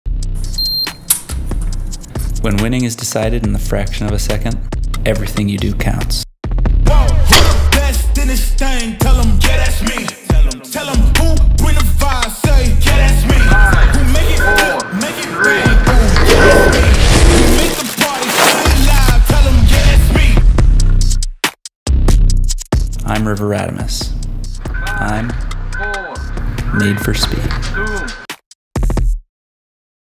River Radamus | Team KASK
We are proud to introduce to the team River Radamus

River is one of the most talented young skiers on the USA Ski Team, winning has always been a habit for him, and now he is ready to step up at the World Cup.

For all downhill, giant slalom, and Super-G athletes there’s just one keyword: speed. This combination of maximizing speed whilst guaranteeing the utmost protection was at the heart of the development process that led us to design the Omega, a high-performance helmet for racers competing in high-speed alpine skiing competitions.